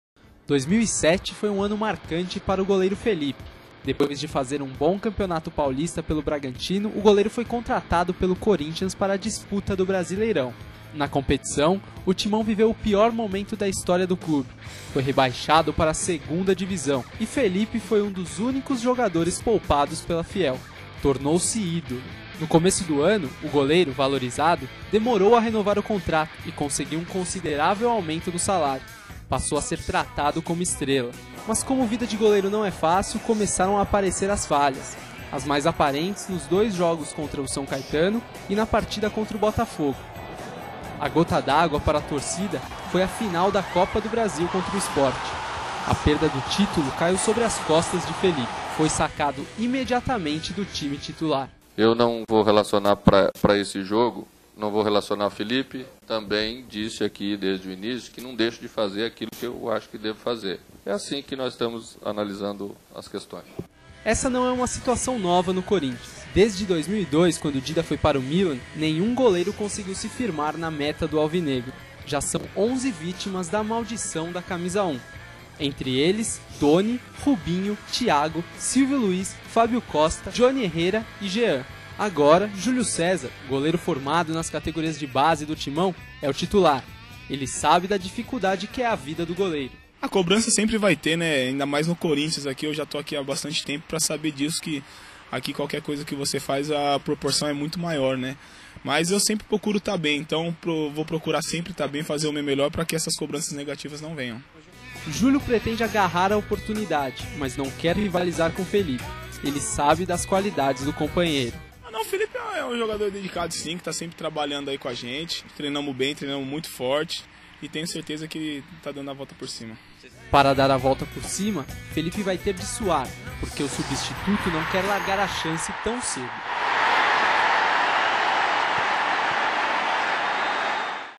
Corinthians: a maldição sobre a camisa 1
Equipe sofre com maldição sobre a camisa 1